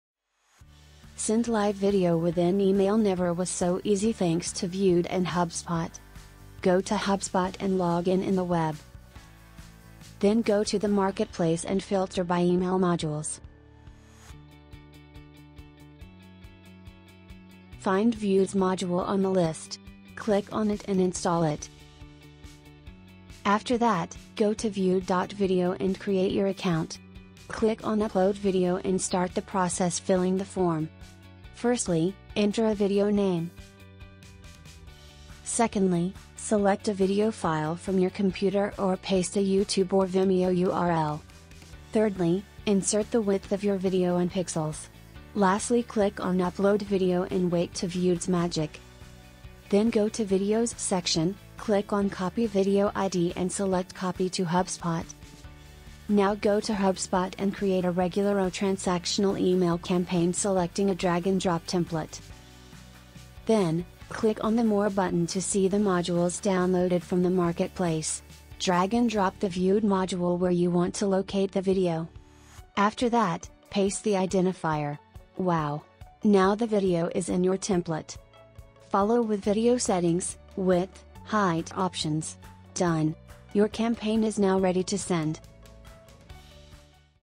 How to send a video by email with HubSpot?
Embedding videos in your emails with autoplay function is very easy and effective, because it will help you to increase your results AMAZINGLY.

Now you can send autoplay videos by email through HubSpot.

Like all technology platforms, HubSpot continues to evolve and modify some of its processes.

Starting in 2022, the process for embedding videos in email using Viewed and HubSpot has changed.

If you already have the Viewed module that was downloaded from the HubSpot Integrations marketplace, follow this tutorial to add videos to your emails, but if you don't have this module, click on the following link here to see another option to send videos by email with HubSpot.